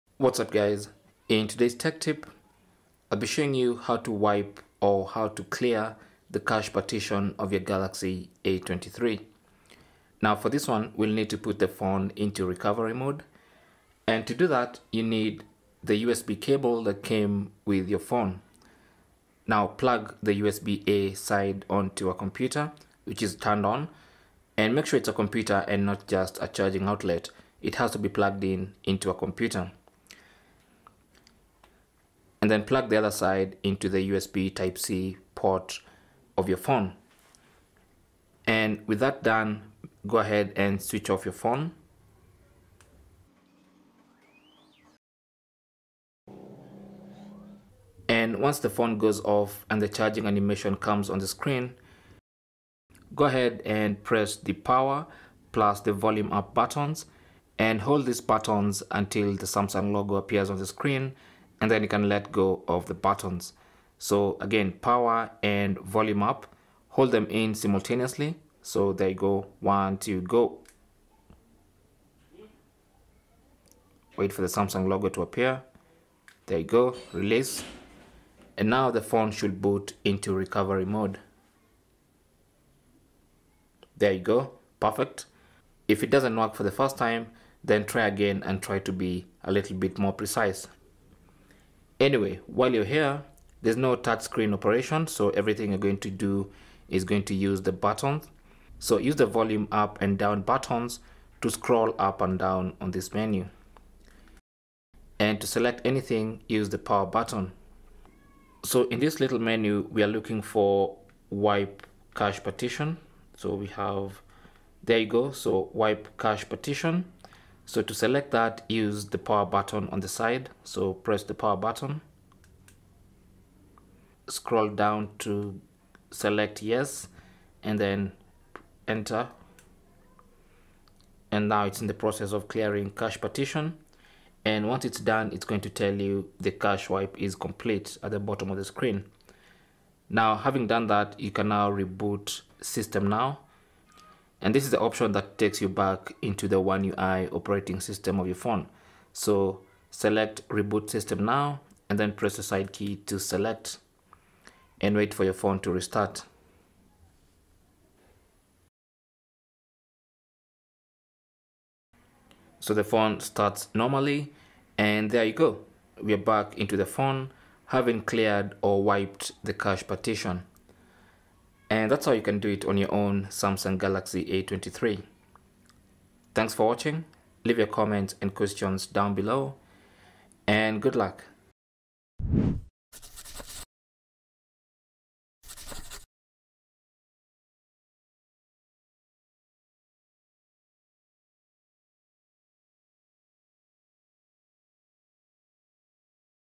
How To Wipe Cache Partition In Samsung Galaxy A23
How to wipe or clear the cache partition on Samsung Galaxy A23. This involves putting your Galaxy A23 in recovery mode and use that mode to wipe clean the cache partition.

How To Wipe Cache Partition:
Put your Galaxy A23 in Recovery Mode
Use the volume rockers to scroll to 'Wipe Cache Partition'
Press power button to select the option.
Confirm.
Reboot System.

Cell Phone Tripod Adapter